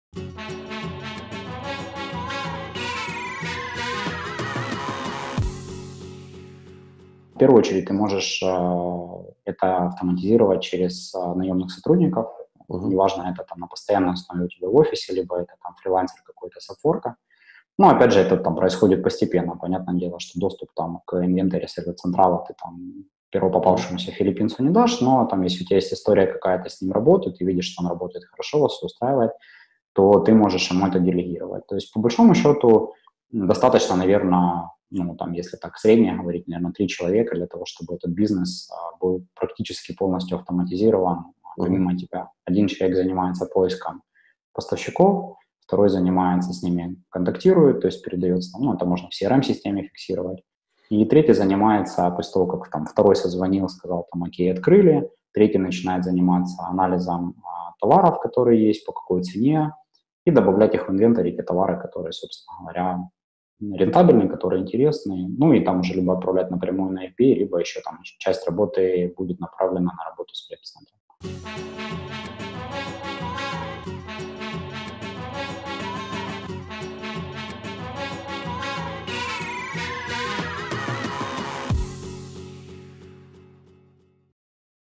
Как правильно автоматизировать Wholesale на Амазон?
sellerboard: Анализ прибыльности для Amazon FBA продавцов + емайл кампании + возмещения за потерянные товары + управление складом + многое другое.

Гость следующего выпуска sellerboard шоу, Геннадий Быков, дает рекомендации как автоматизировать оптовые продажи на Амазон.

Полный выпуск интервью c Геннадием смотрите в следующем выпуске sellerboard шоу!